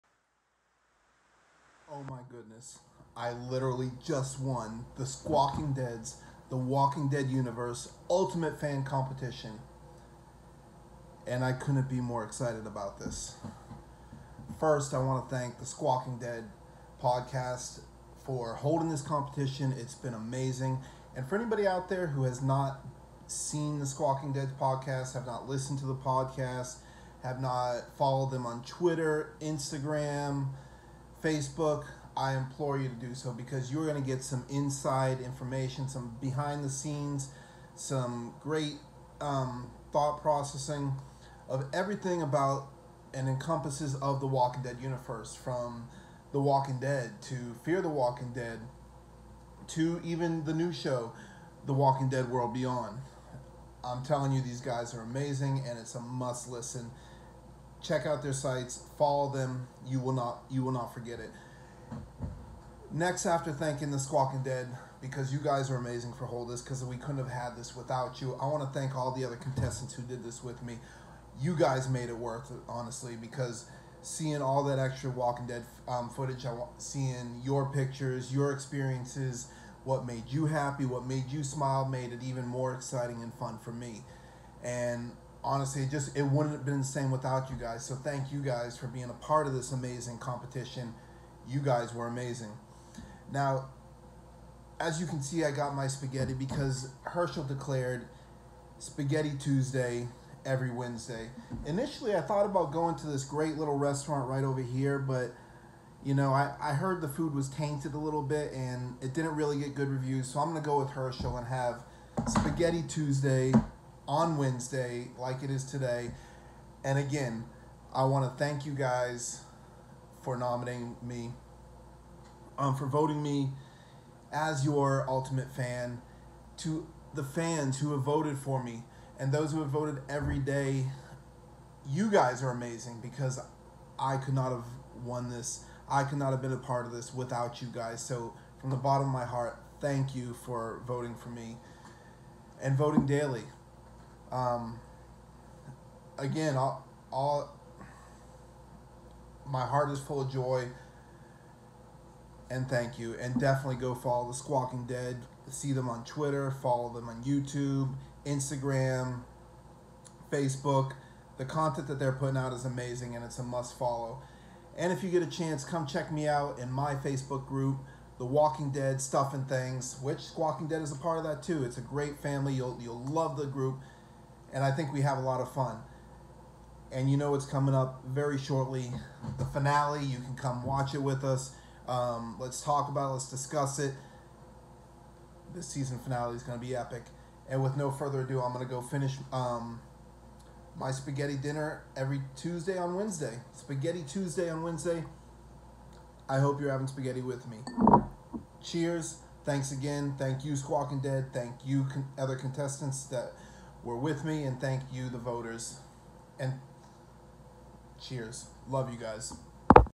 Squawkingdead’s TWDUniverse’s Ultimate Fan competition winner
I wanted to share some exciting news with you guys. As you guys know I have been in SQUAWKING DEAD’s TWDUniverse UltimateFan competition It has been recently announced that I have won the competition and I wanted to take a moment to thank you guys. To all of you that have supported me voting everyday, thank you. To the other competitors thank you. I have enjoyed watching your posts and getting to know you guys. You’re all amazing individuals and your excitement & enthusiasm made this journey so much fun. To the squawkingdead thank you for holding this amazing event. Make sure you follow these guys on Facebook, Instagram, twitter, youtube, iHeartRadio etc. They always bring awesome content and you guys will love them. I love my TWDFamily